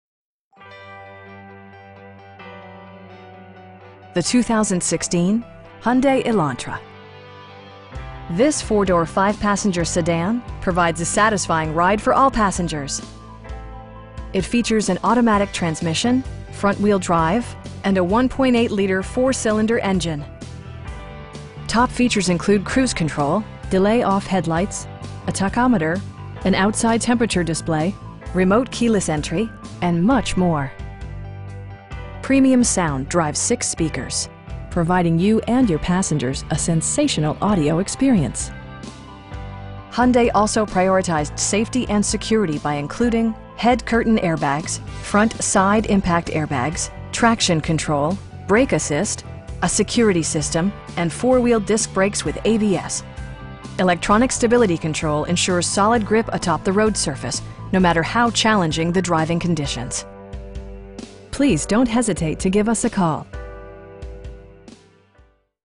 2016 Hyundai Elantra SE in National City, CA 91950
Frank Hyundai
3150 National City Blvd.

The 2016 Hyundai Elantra. This 4 door, 5 passenger sedan provides a satisfying ride for all passengers. It features an automatic transmission, front-wheel drive, and a 1.8 liter 4 cylinder engine. Top features include cruise control, delay-off headlights, a tachometer, an outside temperature display, front bucket seats, power windows, remote keyless entry, and much more. Premium sound drives 6 speakers, providing you and your passengers a sensational audio experience. Hyundai also prioritized safety and security by including: head curtain airbags, front SIDE impact airbags, traction control, brake assist, a security system, and 4 wheel disc brakes with ABS. Electronic stability control ensures solid grip atop the road surface, no matter how challenging the driving conditions. We pride ourselves on providing excellent customer service. Please don't hesitate to give us a call.